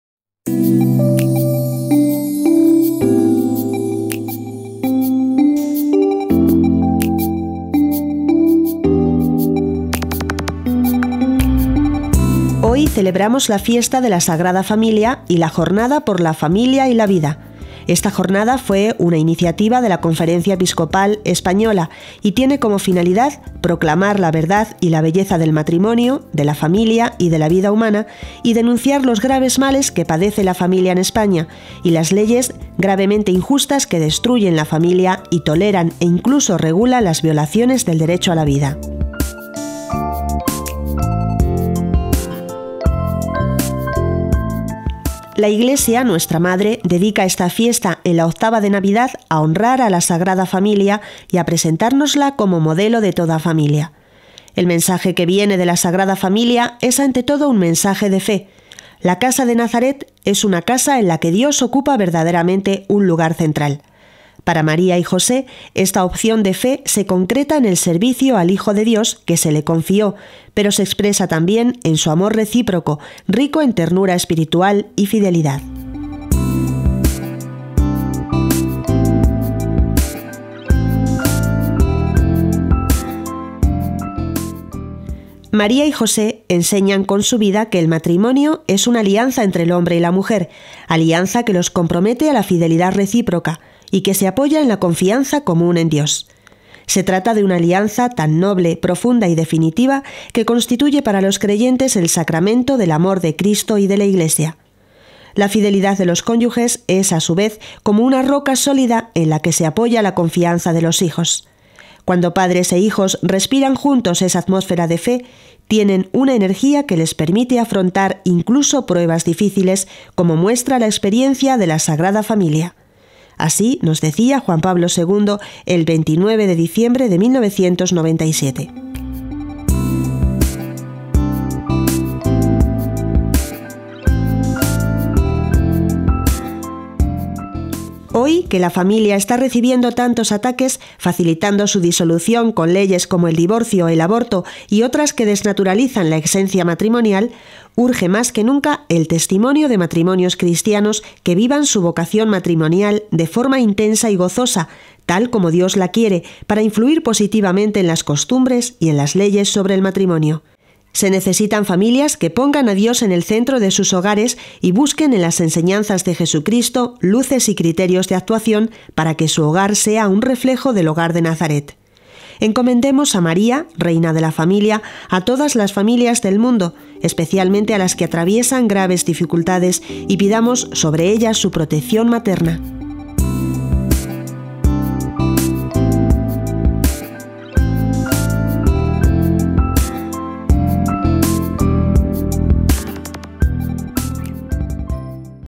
¿Qué santo es hoy?: La Sagrada Familia
La Iglesia, nuestra madre, dedica esta fiesta, en la Octava de Navidad, a honrar a la Sagrada Familia y a presentárnosla como modelo de toda familia. El mensaje que viene de la Sagrada Familia es ante todo un mensaje de fe: la casa de Nazaret es una casa en la que Dios ocupa verdaderamente un lugar central. Para María y José esta opción de fe se concreta en el servicio al Hijo de Dios que se le confió, pero se expresa también en su amor recíproco, rico en ternura espiritual y fidelidad